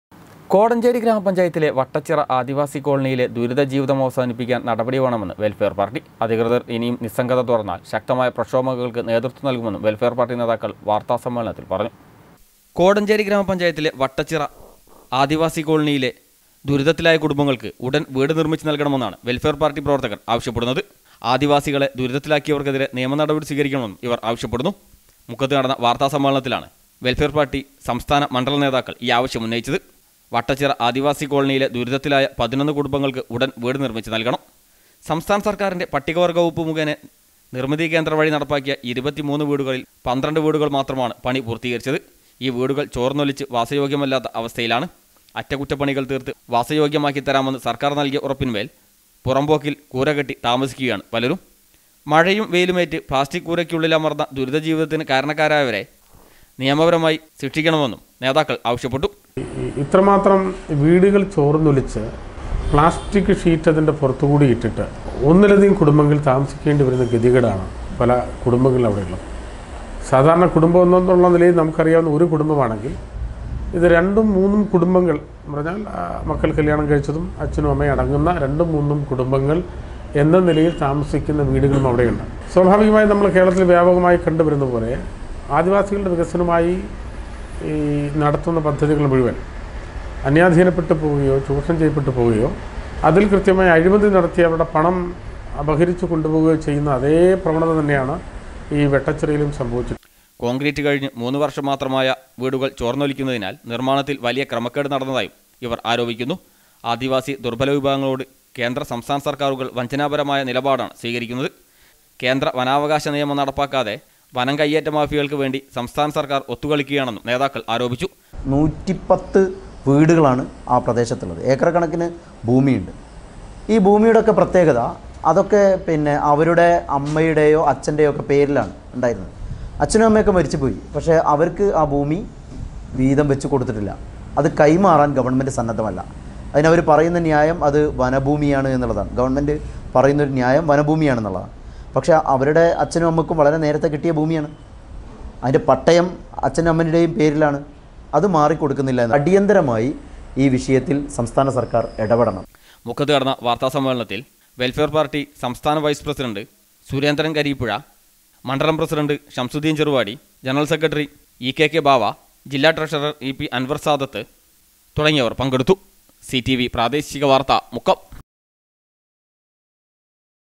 ആദിവാസി കോളനിയിലെ ദുരിത ജീവിതമവസാനിപ്പിക്കാൻ നടപടി വേണമെന്ന് വെൽഫെയർ പാർട്ടി
കോടഞ്ചേരി വട്ടച്ചിറ ആദിവാസി കോളനിയിലെ ദുരിത ജീവിതമവസാനിപ്പിക്കാൻ നടപടി വേണമെന്ന് വെൽഫെയർ പാർട്ടി. അധികൃതർ ഇനിയും നിസംഗത തുടർന്നാൽ ശക്തമായ പ്രക്ഷോഭങ്ങൾക്ക് നേതൃത്വം നൽകുമെന്നും വെൽഫെയർ പാർട്ടി നേതാക്കൾ വാർത്താ സമ്മേളനത്തിൽ പറഞ്ഞു